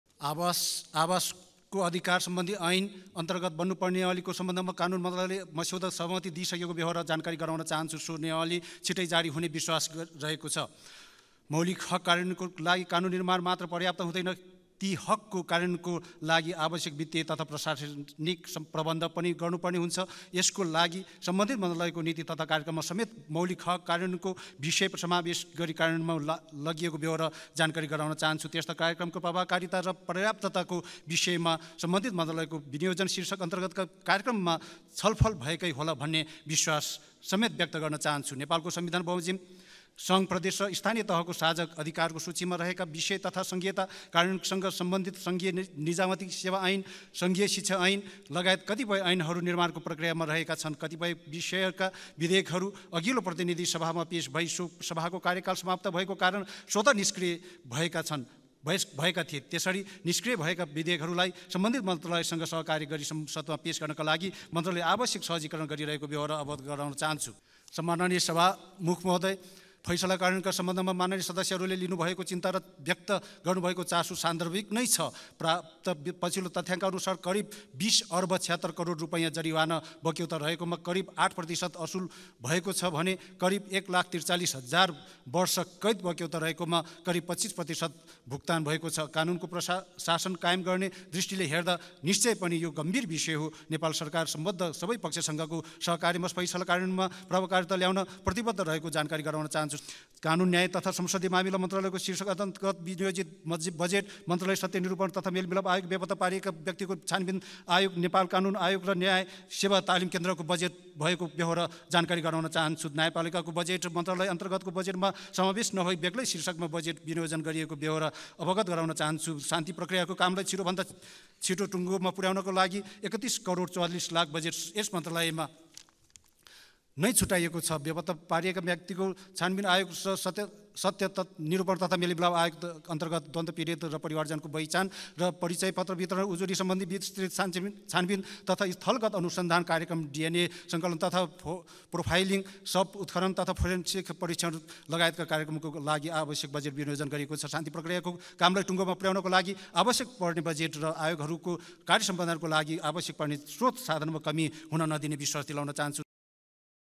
धनराज गुरुङले न्याय सम्पादनलाई छिटो बनाउने विषयमा सरकार गम्भीर छरहेको बताउनुभएको छ ।
कानुन, न्याय तथा संसदीय मामिलामन्त्री धनराज गुरुङले संक्रमणकालिन न्यायसंग सम्बन्धीत बिषयका निष्कर्षमा पुराएर द्वन्द्वपीडितलाई न्याय दिलाउन सरकार प्रतिबद्ध रहेको बताउनुभएको छ ।

मंगलबार प्रतिनिधिसभा बैठकमा आफ्नो मन्त्रालयको बजेटमाथि उठेका प्रश्नको जवाफ दिदै मन्त्री गुरुङले संक्रमणकालिन न्यायसंग सम्बन्धीत बिषयका निष्कर्षमा पुराएर लामो समयदेखि न्यायको प्रतिक्षामा रहेका द्वन्द्वपीडितलाई न्याय दिलाउन सरकार प्रतिबद्ध रहेको बताउनुभयो ।

सत्य निरुपण तथा शान्ति प्रक्रियाको कामलाई छिटोभन्दा छिटो टुङ्गोमा पु¥याउनका लागि रु ३१ करोड ४४ लाख बजेट छुट्याइएको उहाले जानकारी दिनुभयो ।

त्यस्तै उहाले मौलिक हकसम्बन्धी ऐन कार्यान्वयनका लागि बनाउनुपर्ने नियममध्ये दुई नियमावली मात्र बनाउन बाँकी रहेको, निष्कृय विधेयकहरुलाई बढाउनका लागि सम्बन्धीत मन्त्रालयसंग छलफल भइरहेको र बिभिन्न विधेयकहरुमा मन्त्रालयले सहमति प्रदान गरिसकेको जिकिर गर्नुभयो ।

मन्त्री गुरुङले ठूलठूला भ्रष्टाचार काण्डको निष्पक्ष छानबीन गरी अघि बढने तथा सुशासन स्थापना गर्ने लक्ष्यका साथ सरकार अघि बढेको बताउनुभयो ।

उहाले मन्त्रालय न्याय सम्पादनलाई छिटोछरितो र सर्वसुलभ बनाउने विषयमा सरकार गम्भीर रहेको र

छिटोछरितो न्याय सम्पादन गर्न मन्त्रालय आवश्यक गृहकार्यमा समेत जुटेको उल्लेख गर्नुभयो ।